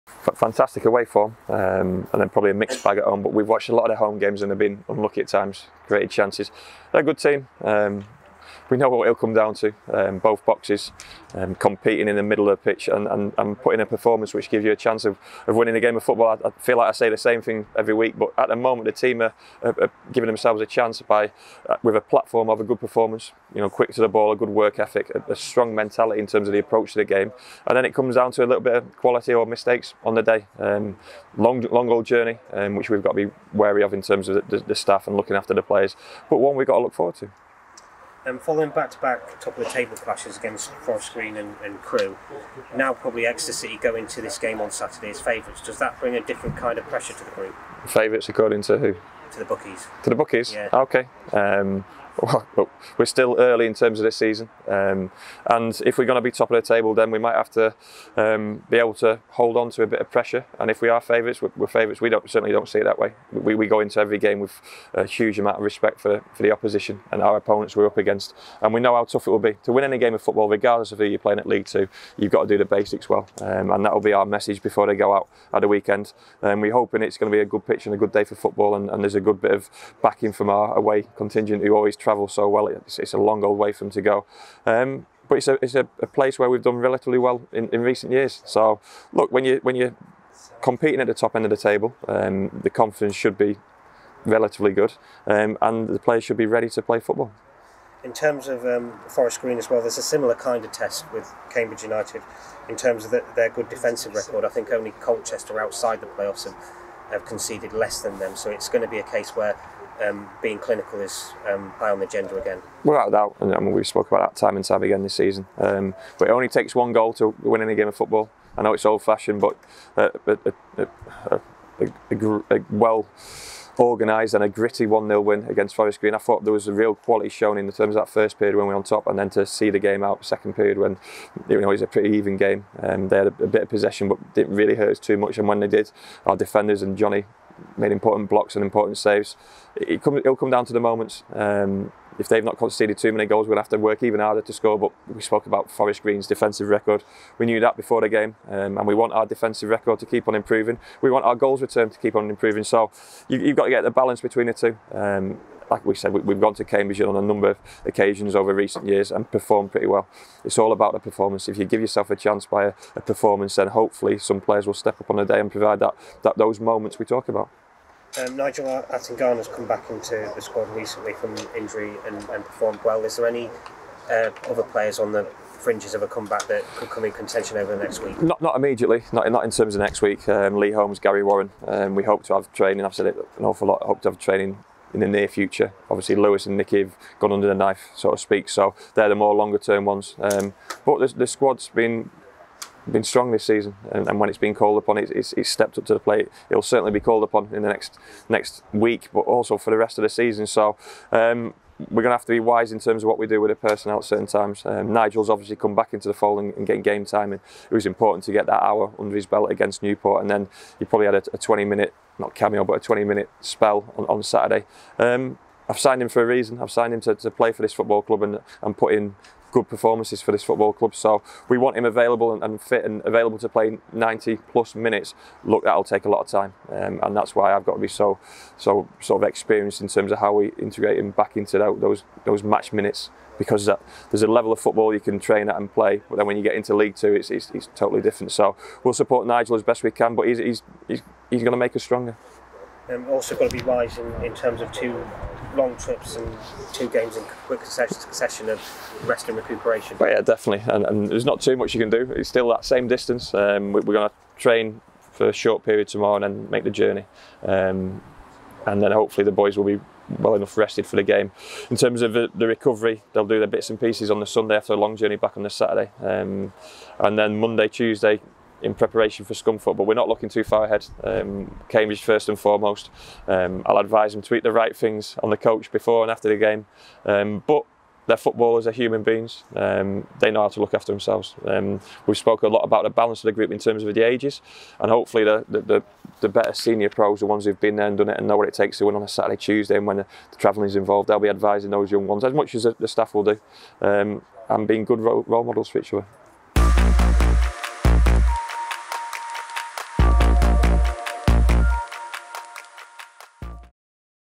💬 Matt Taylor pre-Cambridge United (A) | Exeter City Football Club
🎥💬 "I feel like I say the same every week but at the moment the team are giving themselves the chance with the platform of a good performance."

Matt Taylor on Saturday's match against Cambridge United Football Club at the Abbey Stadium.